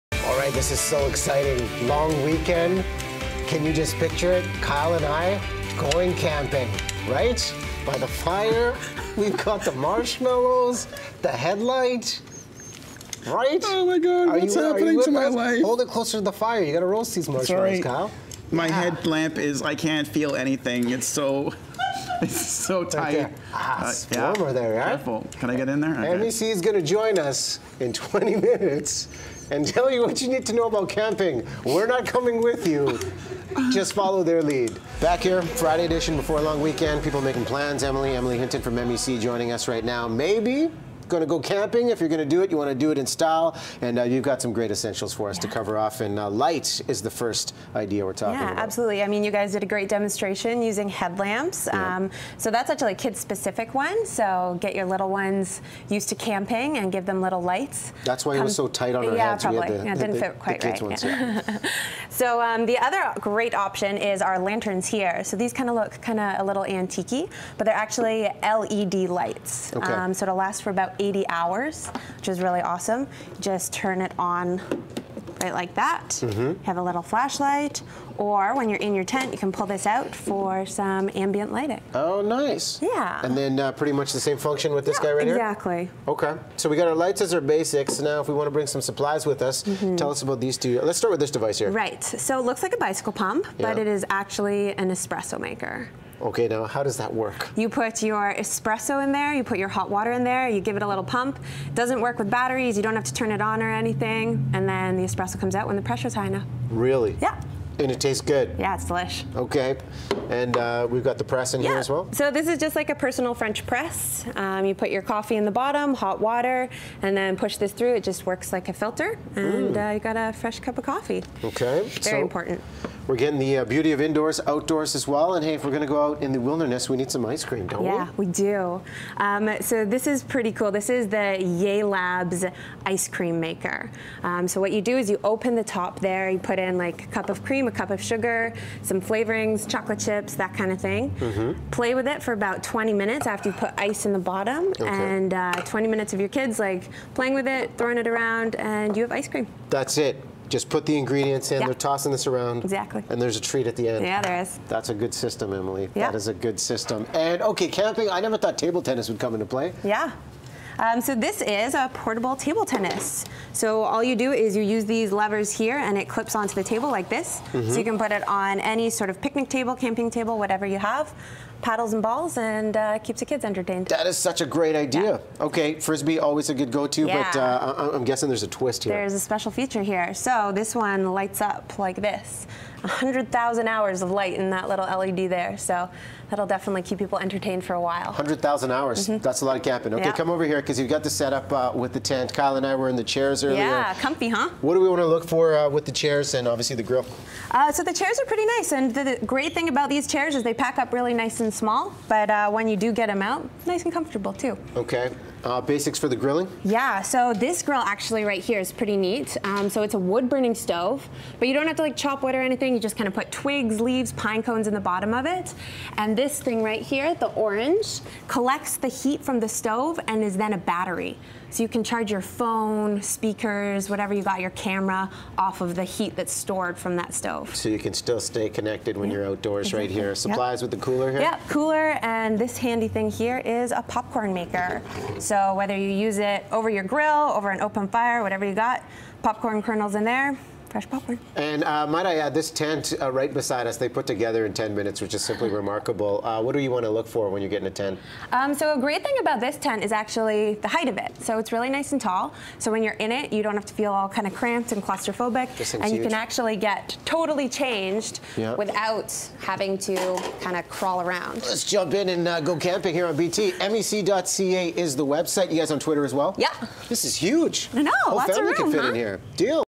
Espresso Machines to Tents: The Latest & Greatest in Camping Gear
Going camping this weekend?  From espresso machines to tents we show you the latest and greatest in camping gear for all budgets.